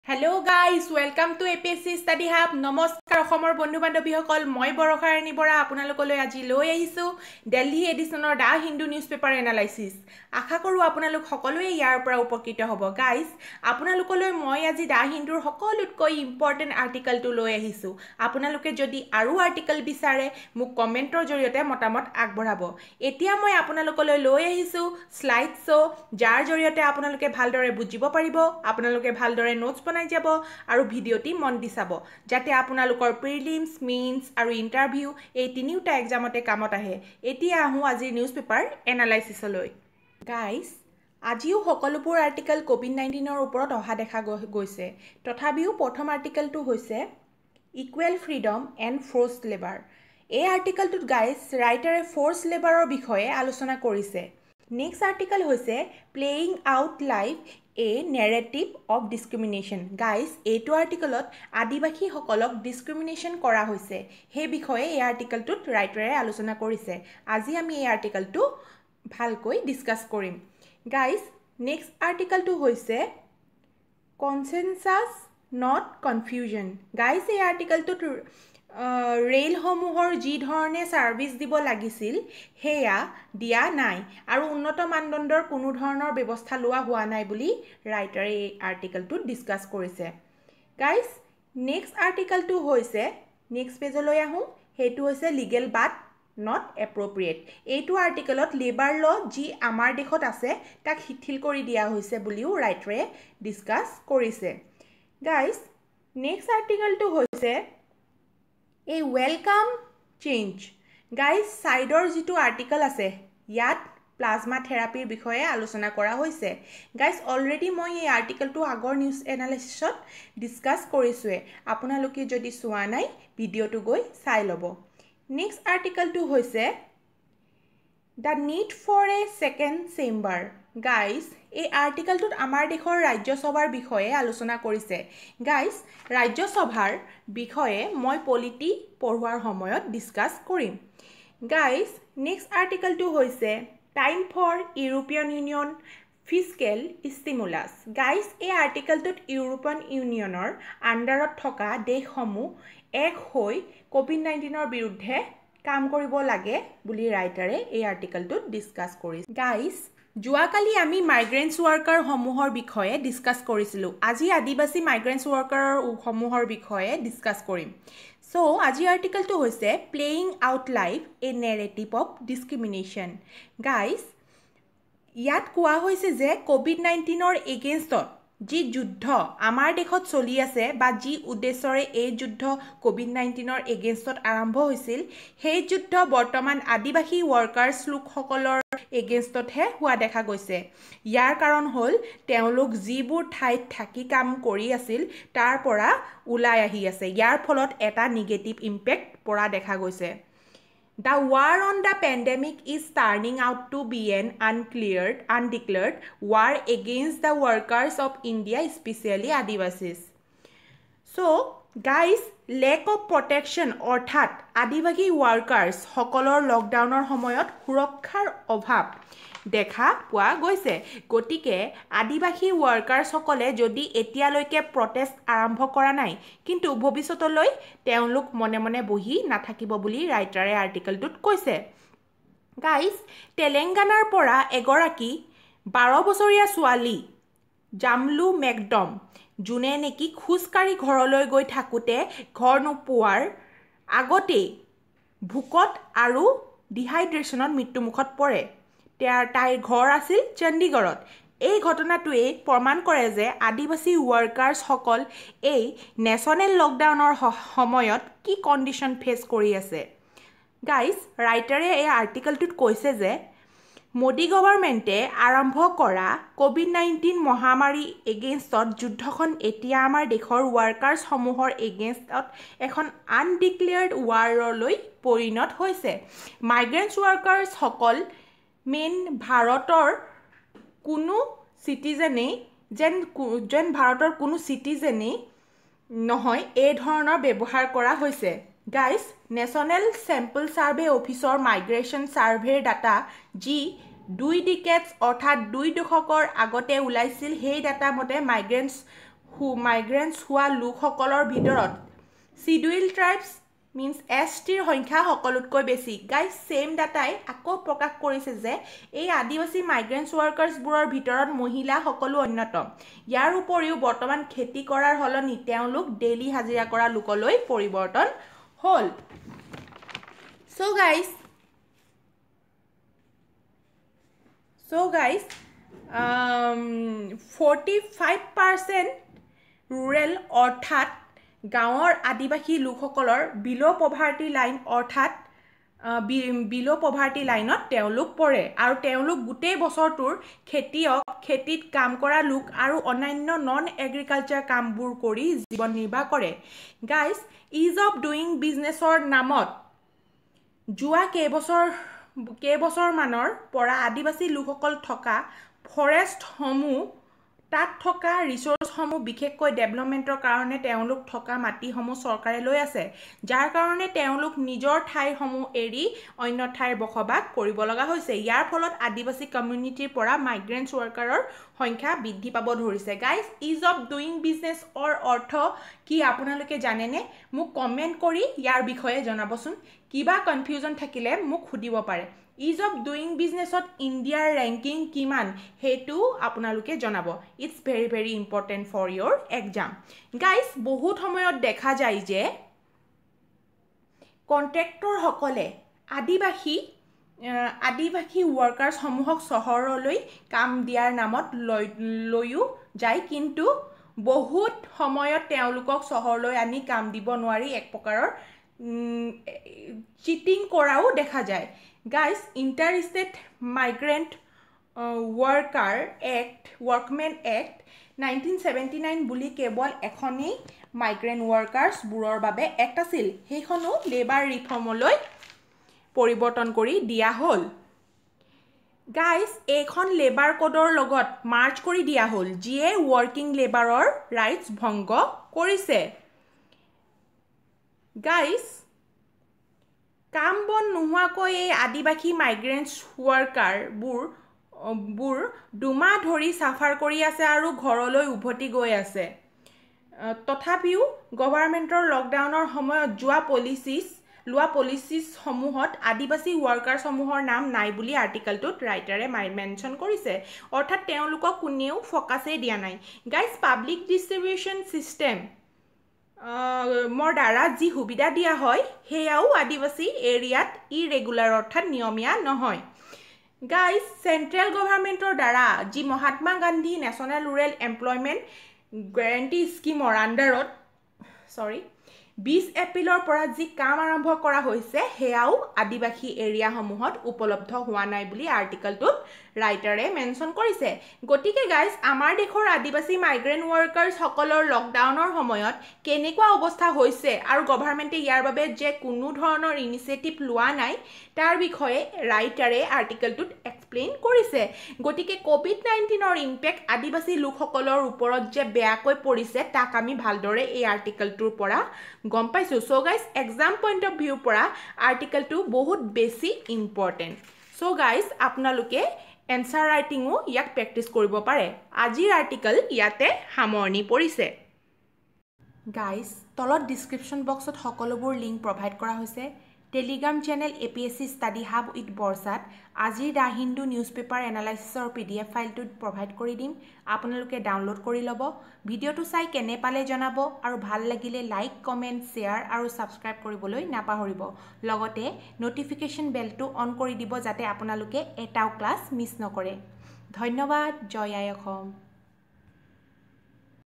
The Hindu Editorial Analysis 12 May 2020||In Assamese by Barsha||APSC||UPSC||EPFO
12 May 2020- The Hindu Editorial Discussion and News paper Analysis in Assamese[APSC/UPSC/IBPS/EPFO]

Social media hendeles-
Telegram channel- Apsc Study Hub with Barsha